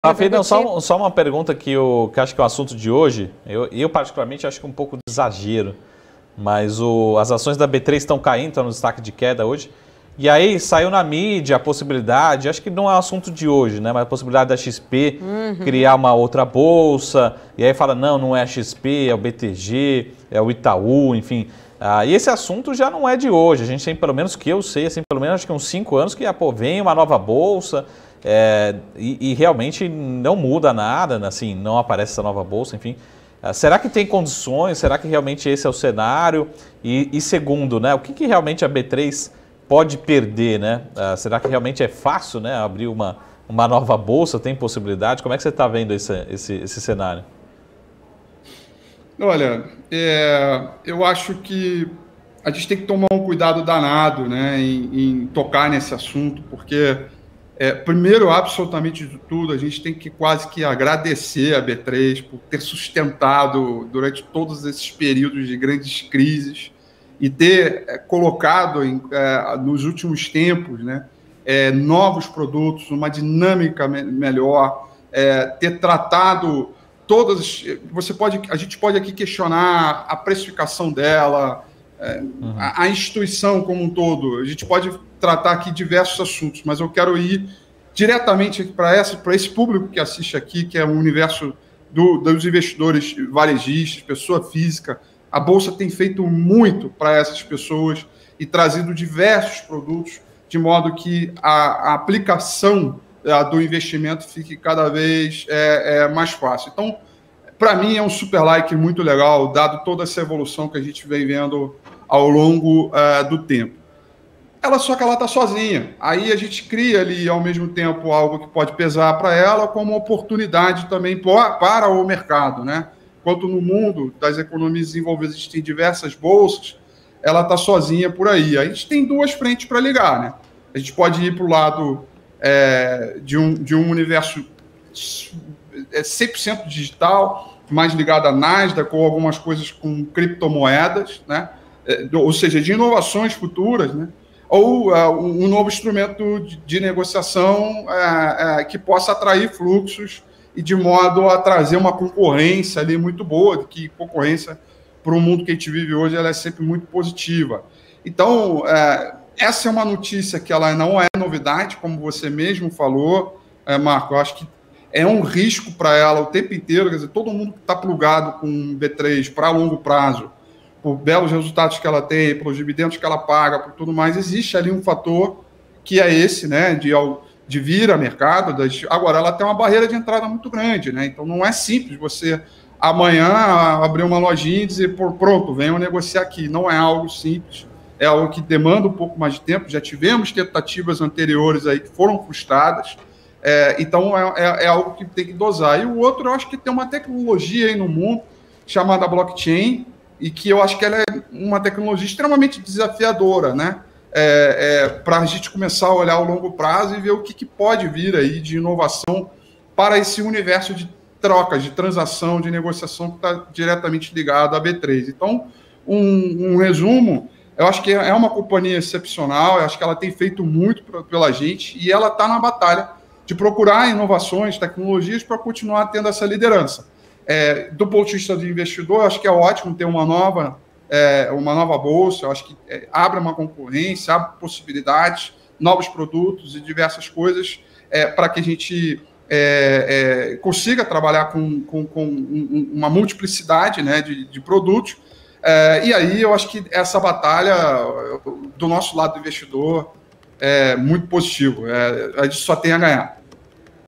Analista da Eleven analisa impactos de nova bolsa de valores (B3)
Raphael Figueiredo, analista da Eleven, comenta reflexos da criação de nova bolsa de valores para o mercado.